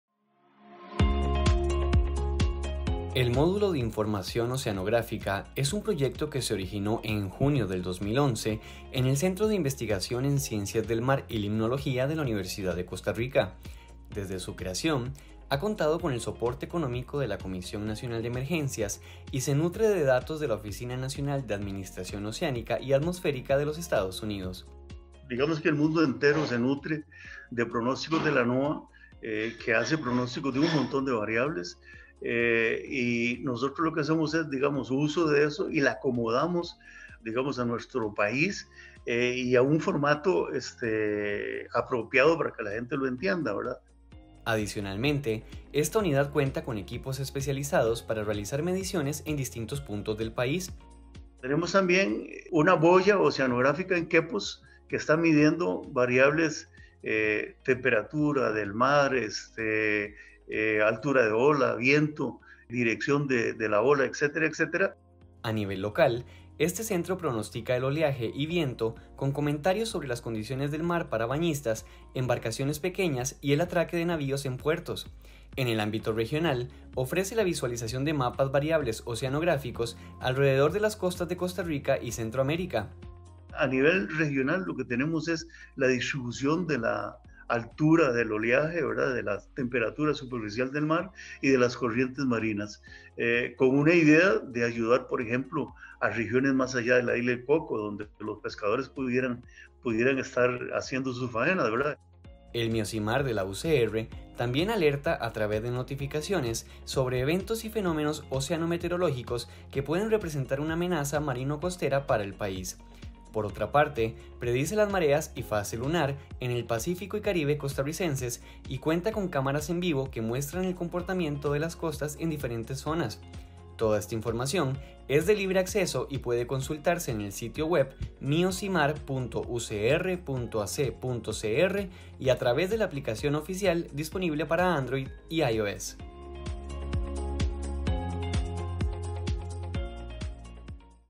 10º Aniversario del MIO CIMAR - UCR
El Módulo de Información Oceanográfica, del CIMAR de la UCR, cumple 10 años de ofrecer información del estado del mar a nivel nacional y en la región.